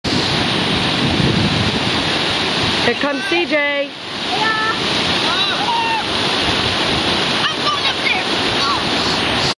Screw Auger Falls
Screw Auger Falls in Grafton, Maine! Brother sliding down some little rocks!!!